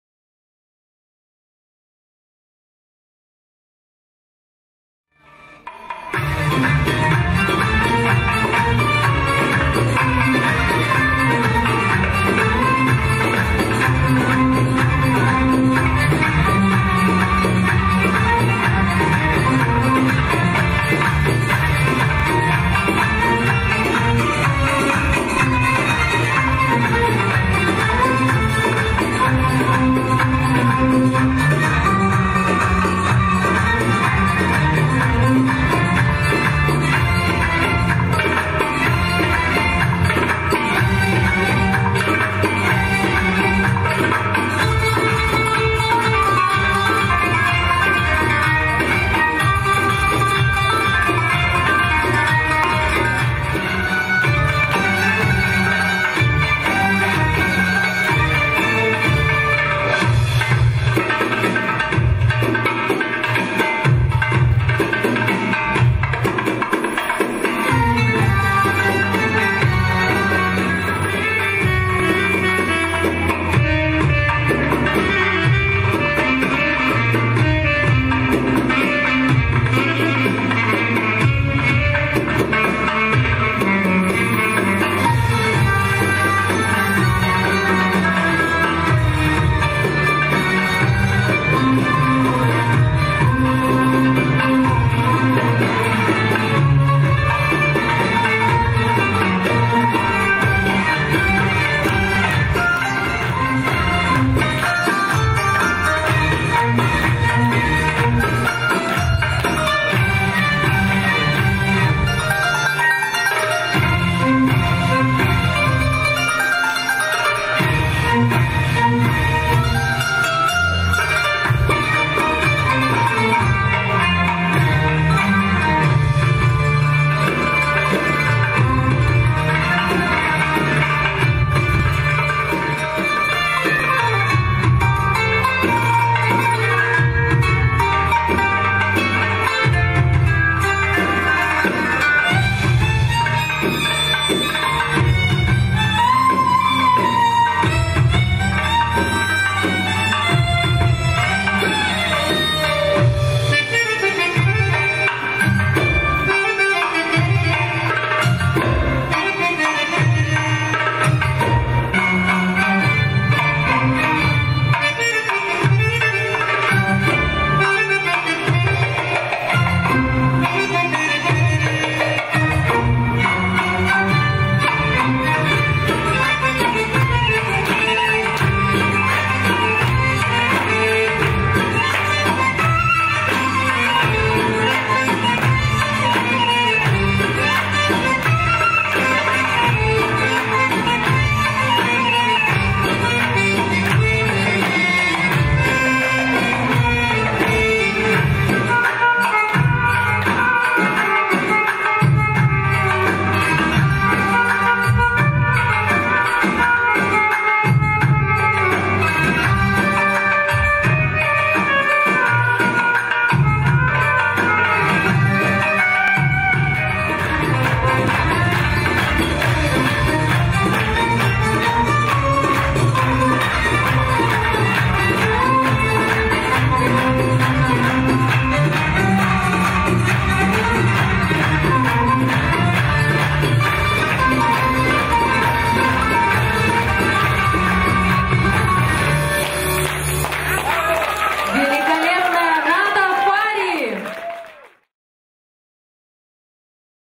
NATA FARI BELLYDANCER RUSSIA ST-PETERSBURG 2017
Nata Fari bellydancer performance 2017 Russia St-Petersburg Eastern Rhythmes festival organizer Yassmin Al-Asuan